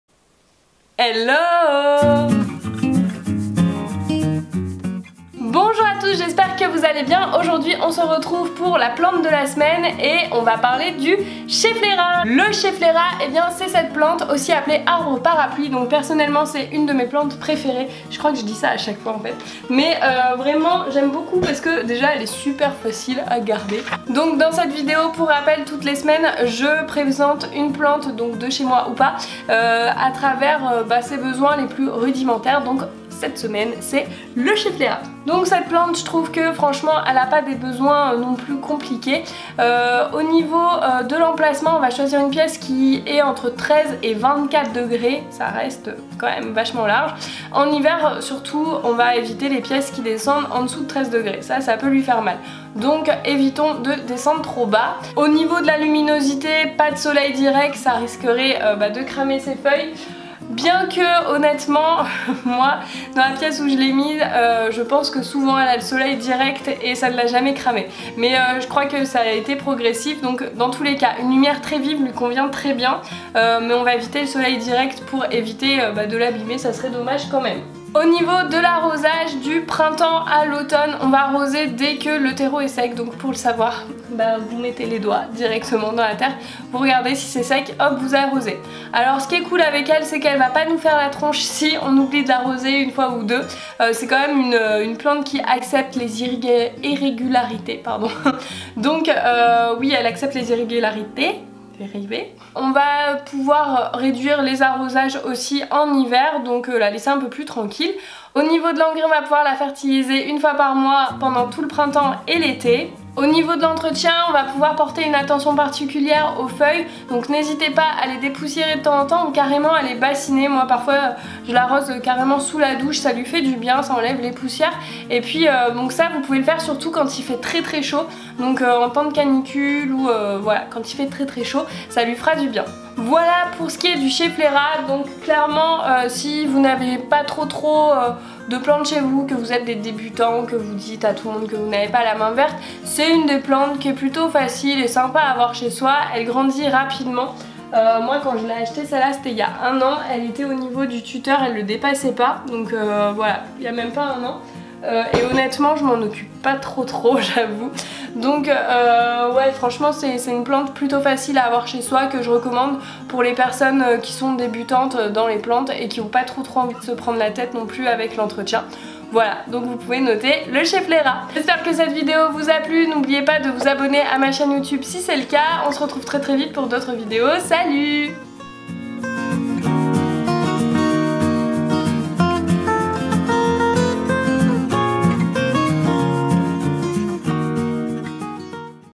🌿 Le schefflera : conseils d'entretien, exposition
Chaque semaine, retrouvez la présentation d'une plante ! Cette semaine : Le schefflera !
Une plante robuste et facile à entretenir, qui pousse rapidement et convient très bien aux plant lover débutant, pour commencer une collection de plantes vertes d'intérieur ou une jungle urbaine !
Cette plante est également appelé arbre parapluie.

Si ce genre de vidéos te plait n'hésite pas à me le dire en commentaire !